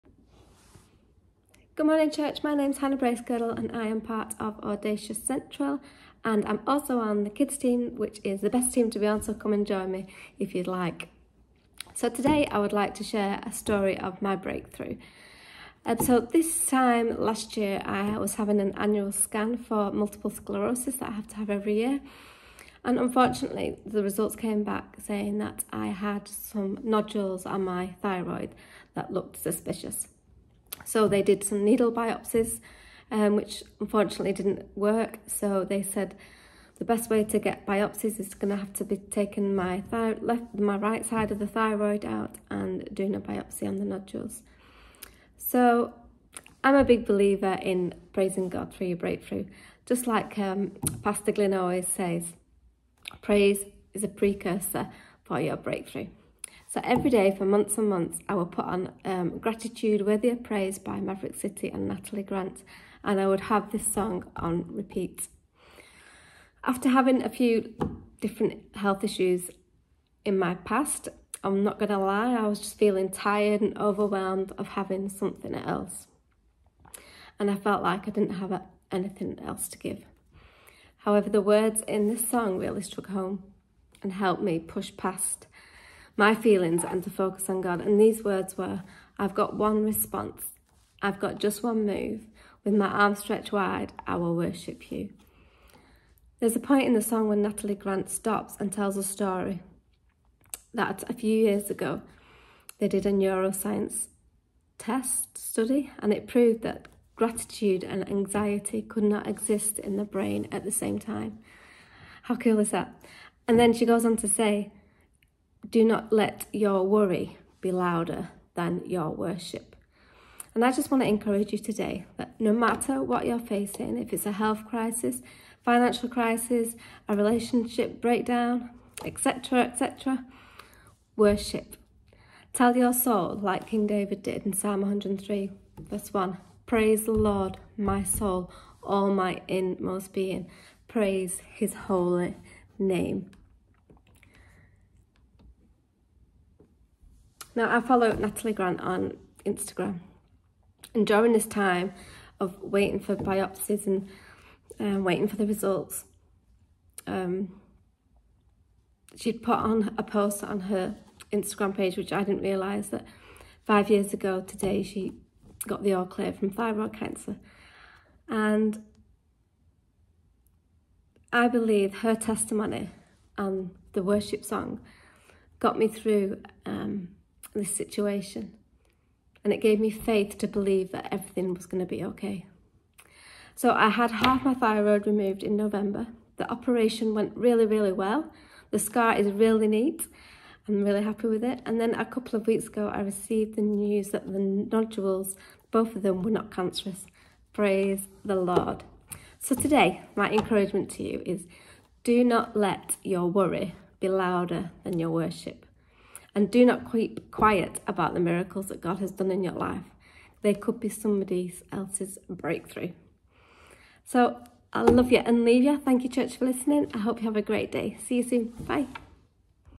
My Miracle - !Audacious Devotionals - Monday 27th February 2023
Today we continue our series of  'My Miracle'  devotions where we hear from a number of people who share with us their story of change and provision after God had answered their prayers.

We continue our series with Hannah Bracegirdle who attends our Central Manchester Campus.